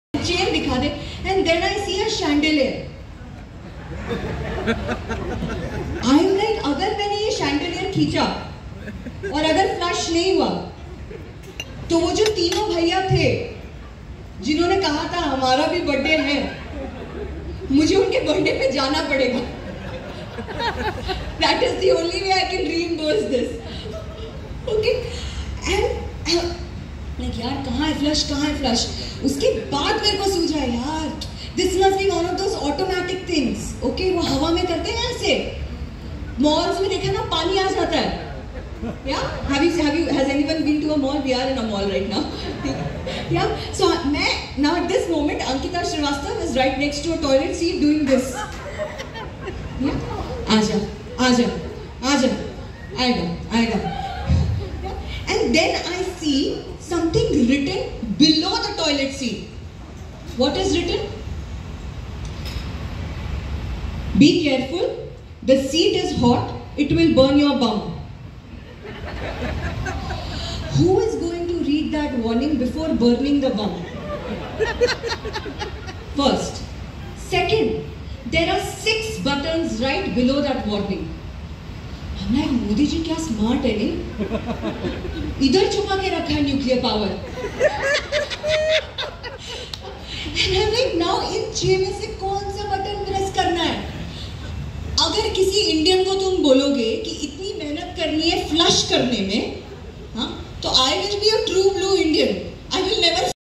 Ankita Shrivastav Live stand up comedy in Bangalore on 12th Nov 2022
Loved the performance by Filmy Chokri ( Ankita Shrivsatava) in Fox in the Field in Prestige Shantiniketan mall in Whitefield on the 12th of Nov 2022. This is just a 1.5 minute video from the audience. Follow her channel @filmychokri